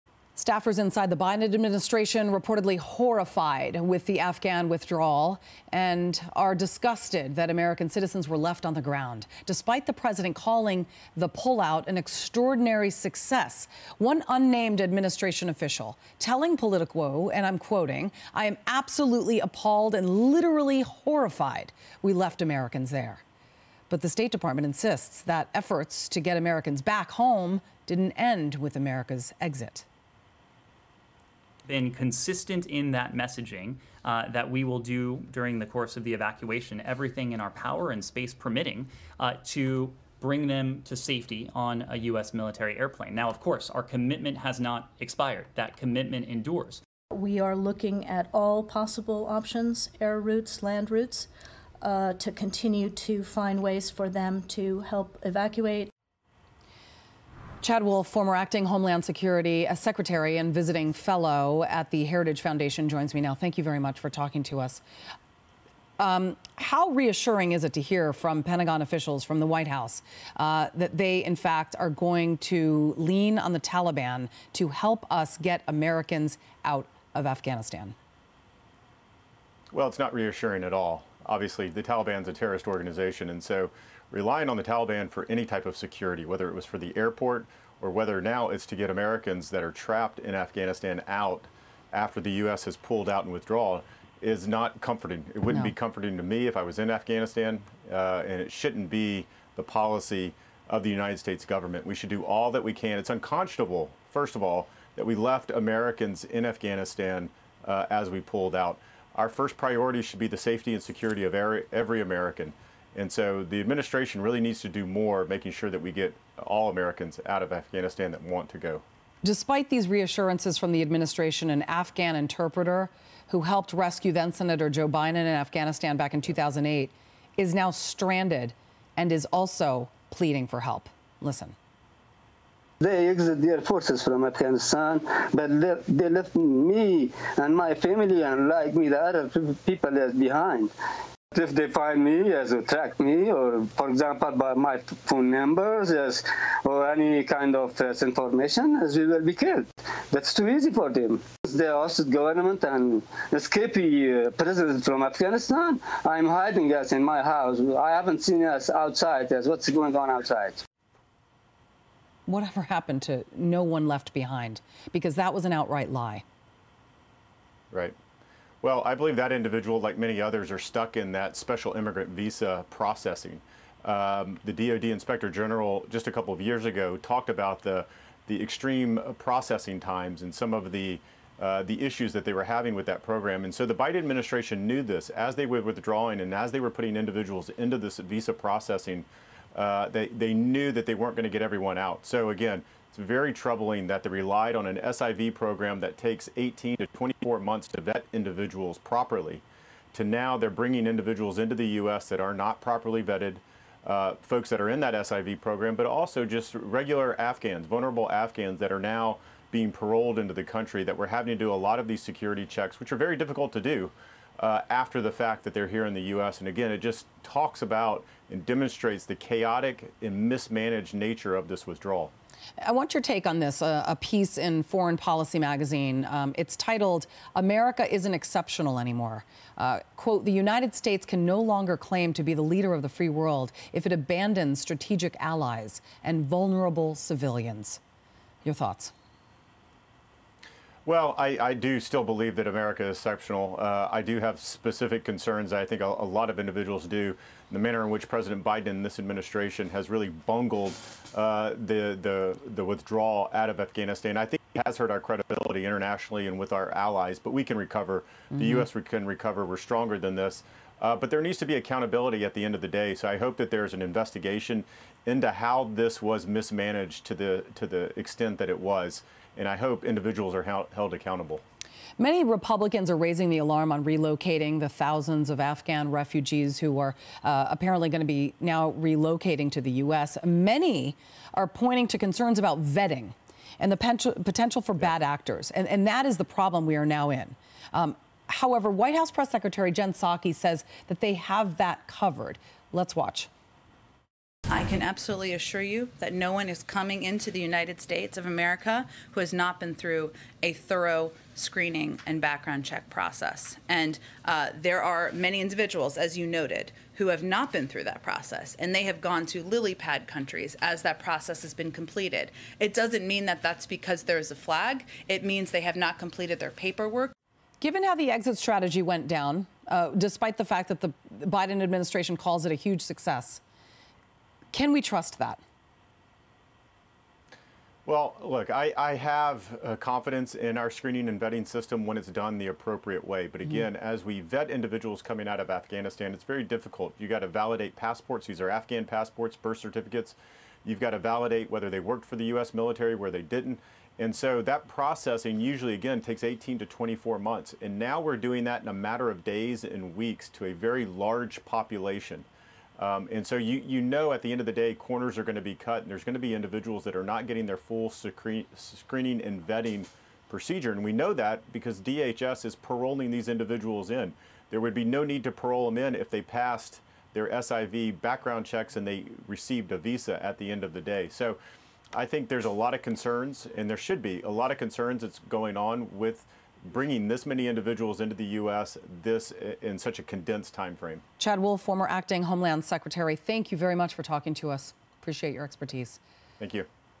Chad Wolf calls for investigation into Biden's handling of Afghanistan withdrawal
Heritage Foundation visiting fellow Chad Wolf discusses leaving Americans and Afghans behind in Afghanistan and the Special Immigrant Visa program used by refugees.